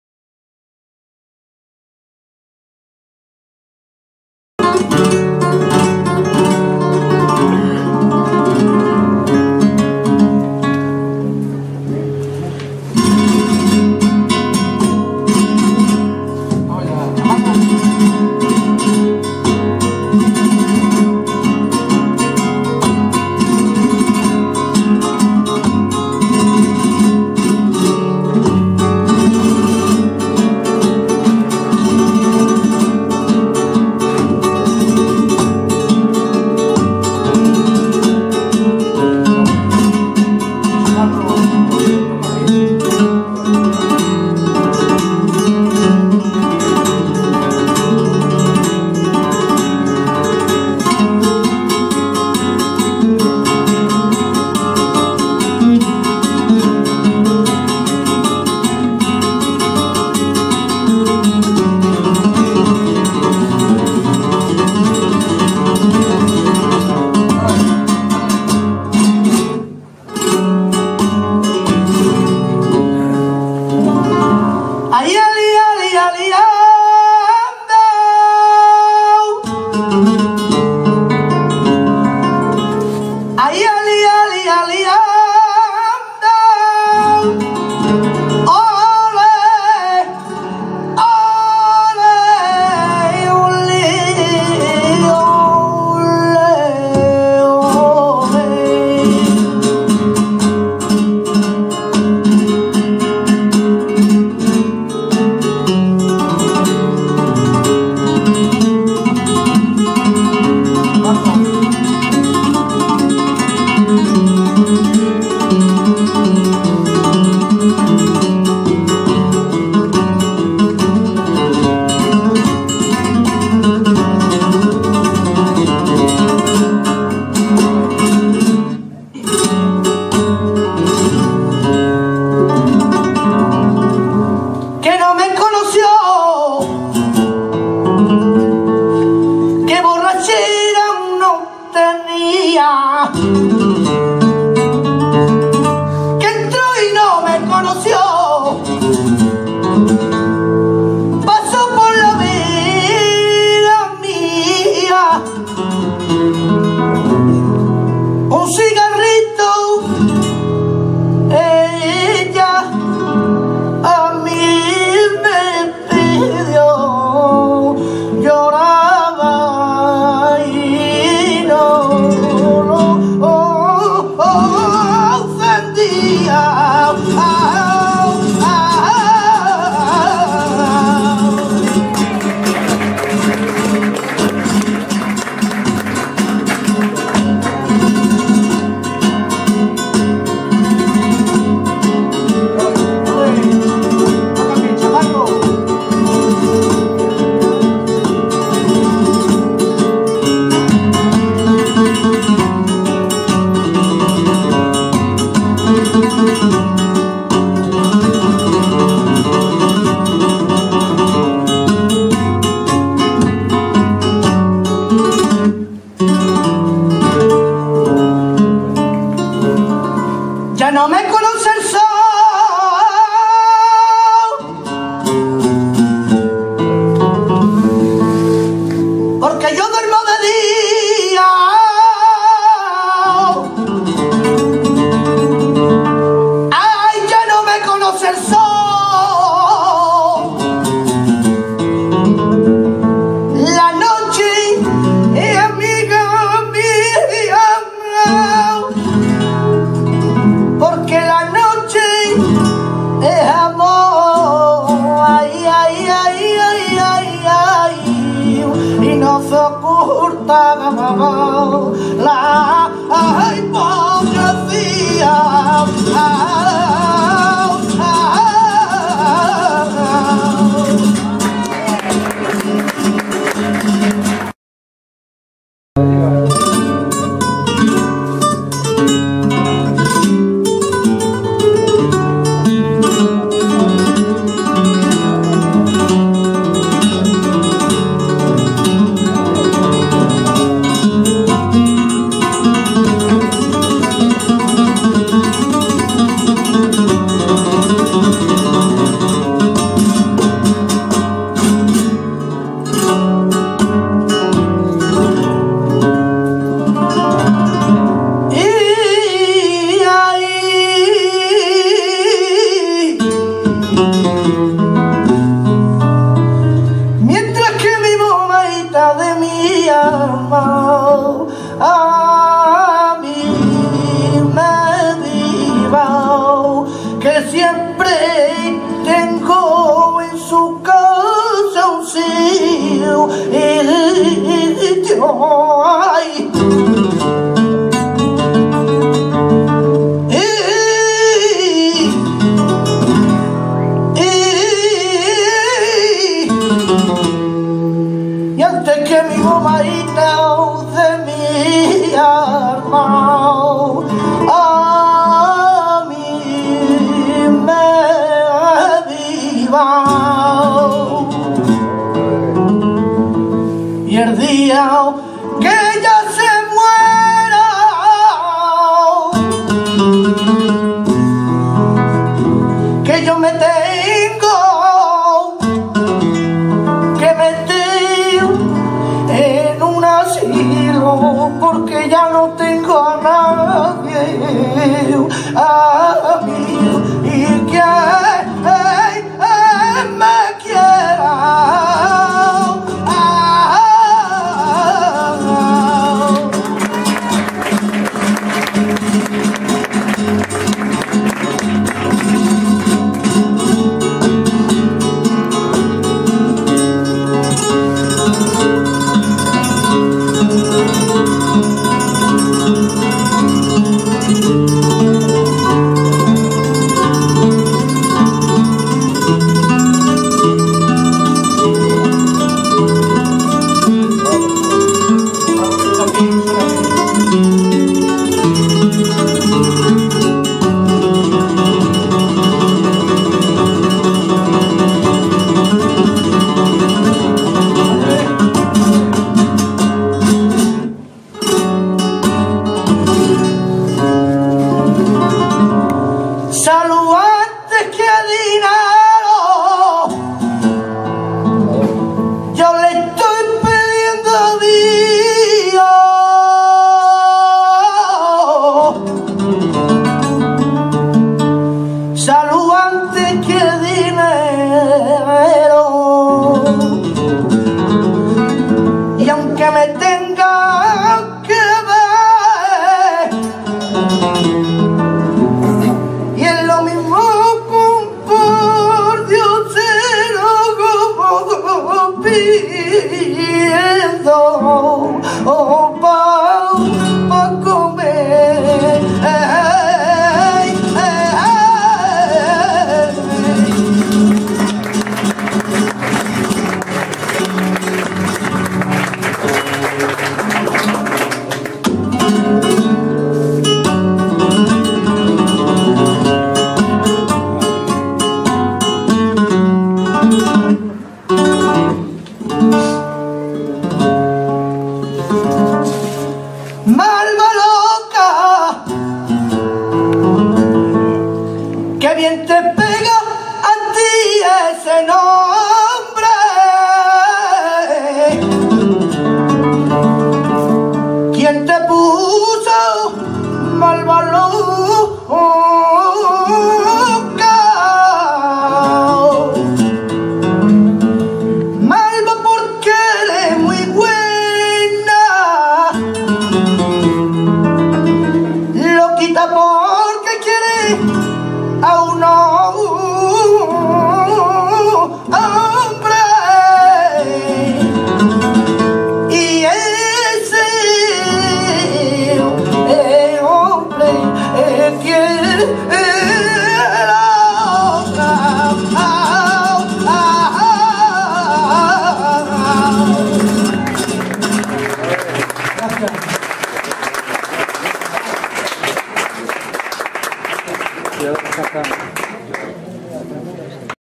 BENALUP-CASAS VIEJAS XXXVII Concurso N.Cante Flamenco 1ª NIÑO DEL CHAPARRO Fandangos
Al toque: Chaparro